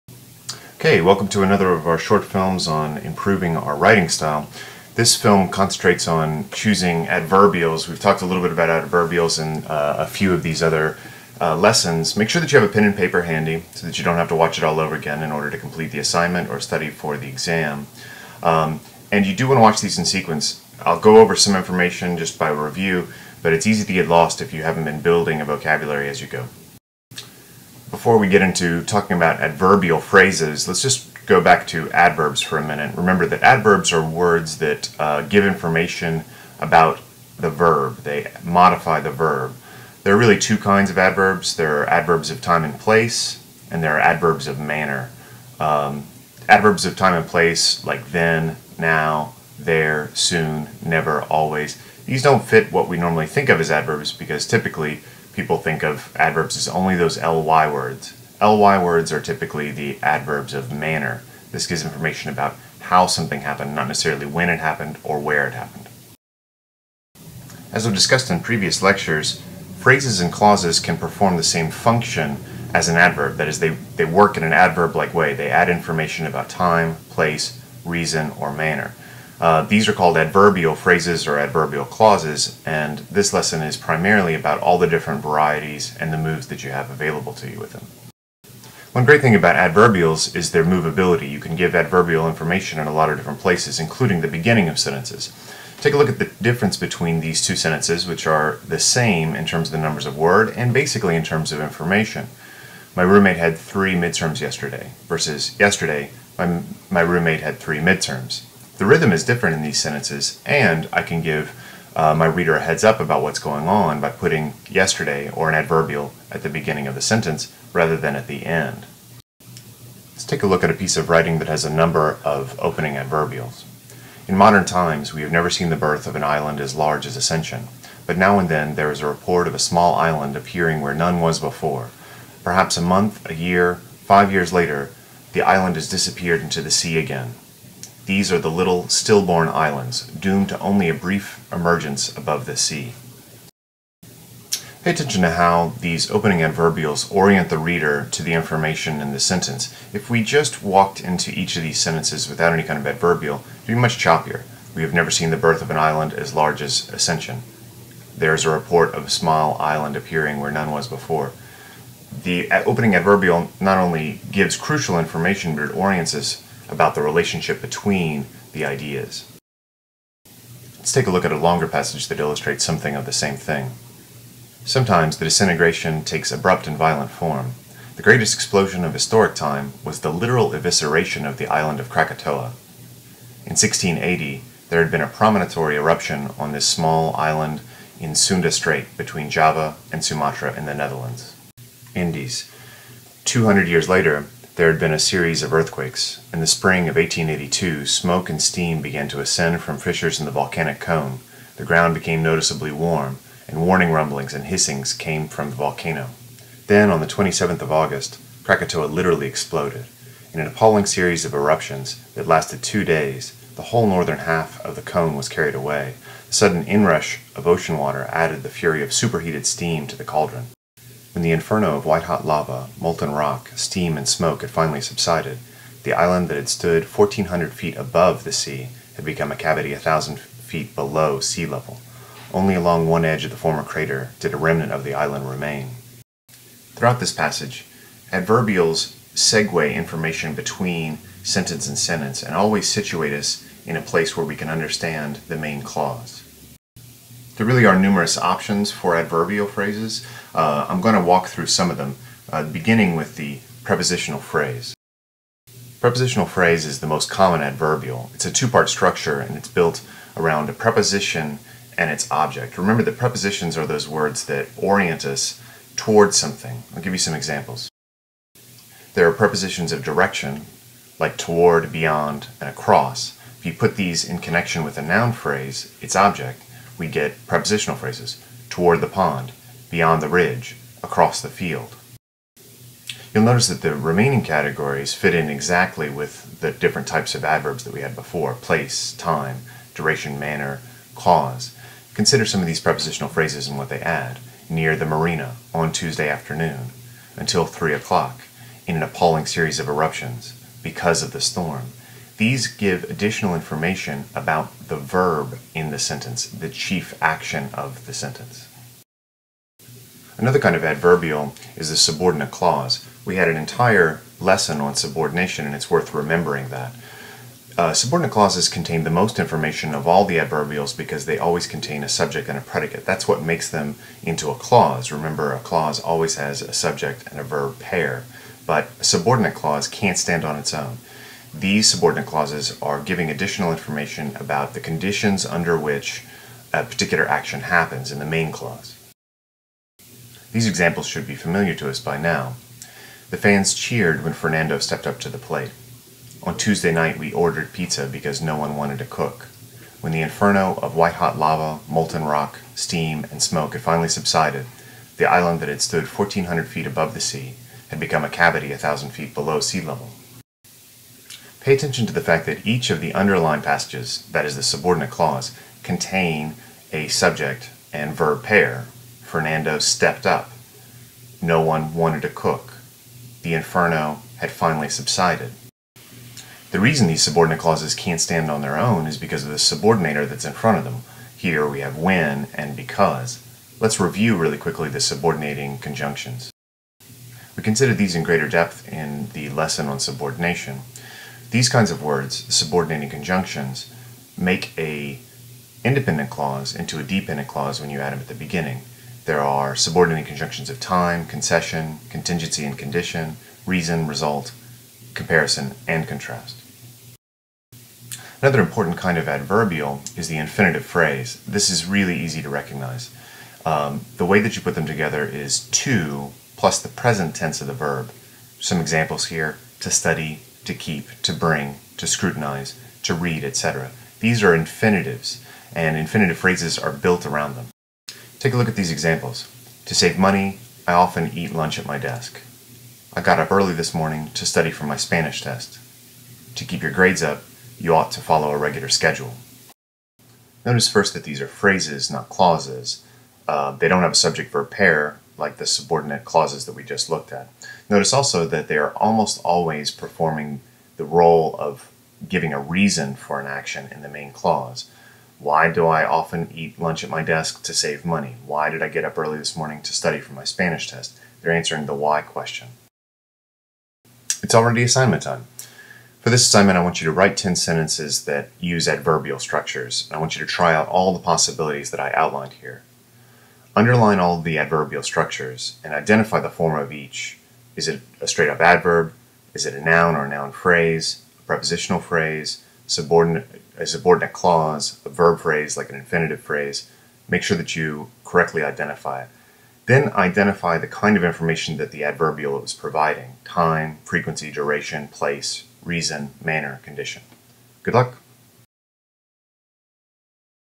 WRITING STYLE 7: Choosing Adverbials
This is the seventh lesson in Dr. Chandler's online guide to writing. These lectures are deeply indebted to Martha J. Kolin and Loretta S. Gray's excellent guide Rhetorical Grammar. They also make use of instructive materials found in The Well-Crafted Sentence by Nora Bacon and They Say/I Say by Gerald Graff and Cathy Birkenstein.